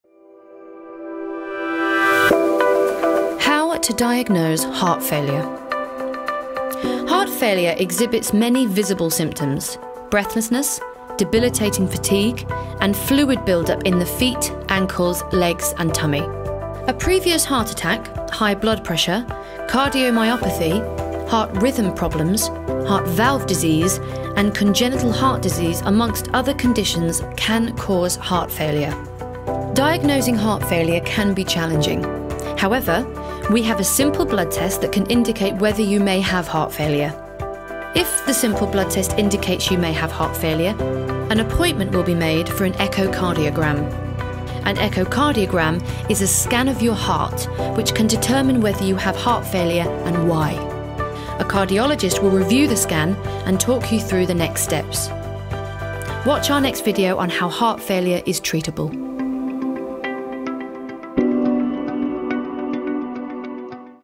How to Diagnose Heart Failure - Educational Video
A defining series of videos, talking about Heart Failure.
Why is it important to have some knowledge of heart failure? What do you need to know about heart failure? What sort of treatments are available? Who should be part of your medical team? How can you help yourself to live better with heart failure, and is it treatable or not.
These are some of the common questions that have come up time and time again. Our series of videos breaks it down to a digestible, snappy series of episodes that answer fundamental questions.
If you want to learn anything else, then there's always our
First up Series 1, Episode 3 - How to diagnose Heart Failure?